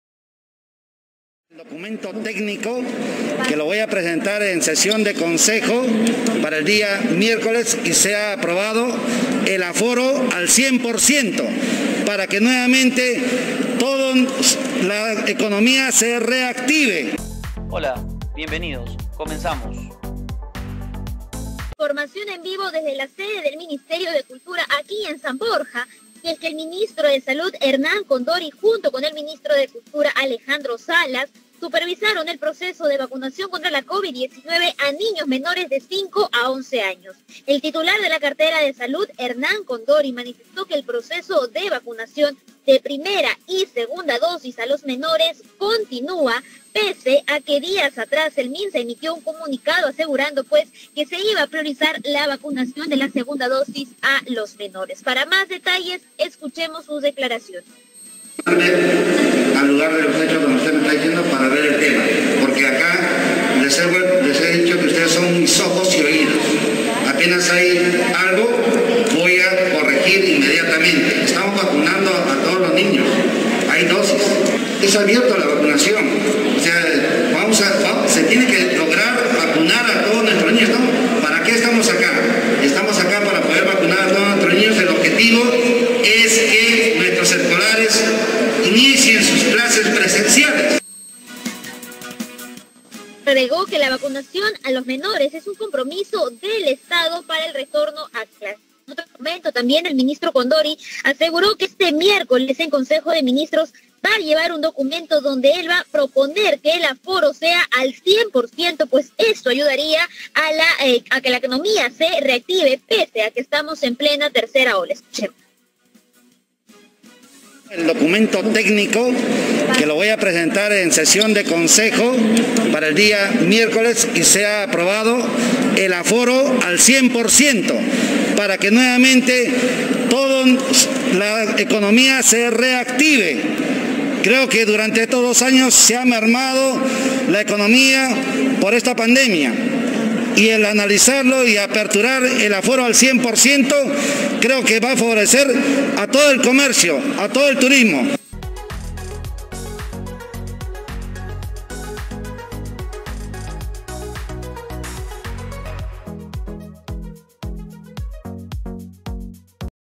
¡ATENCIÓN!..EL MINISTRO DE SALUD PROPONDRÁ AMPLIAR AL 100% EL AFORO |¿HABRÁ CAMBIOS EN LOS COLEGIOS?
El ministro de Salud  anunció que el próximo miércoles presentará ante la sesión del Consejo de Ministros una propuesta para que el aforo de los espacios cerrados y abiertos sea del 100%, a fin de impulsar la reactivación económica del país.
¿Cuáles tu opinion respecto a esta noticia? Déjame tus comentarios...